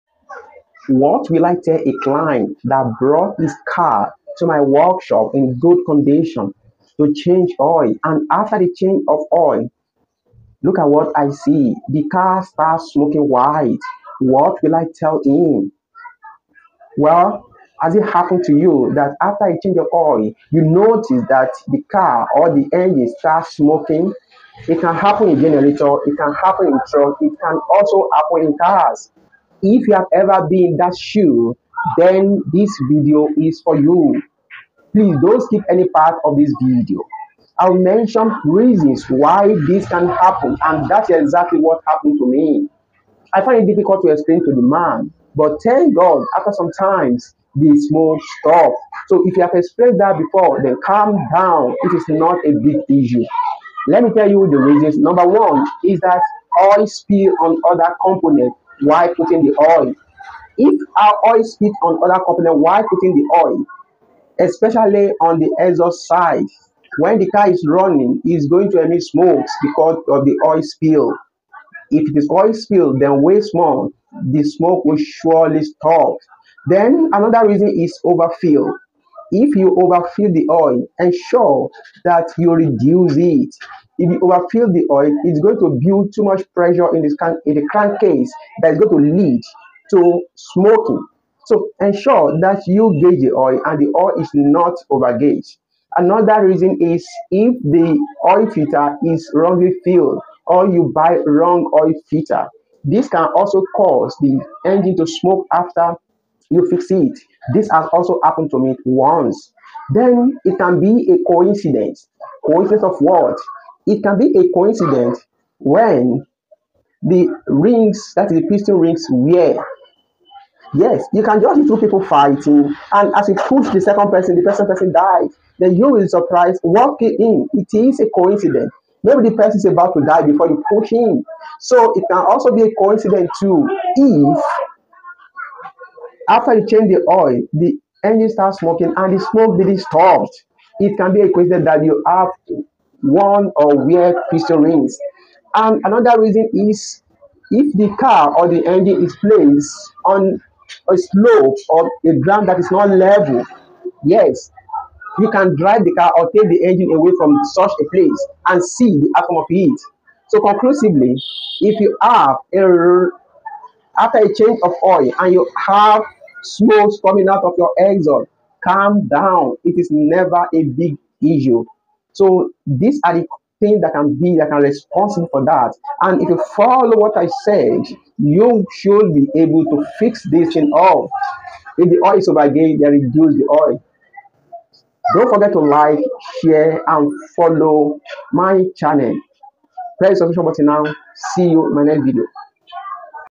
Why Engine Smoke After Change Of Oil
Why does the engine smoke after change of oil?

Many people have seen this, that after changing a diesel engine oil the engine start smoking.
The reason for car smoking after oil change is explained in the video. it can also be that generator start smoking after of change.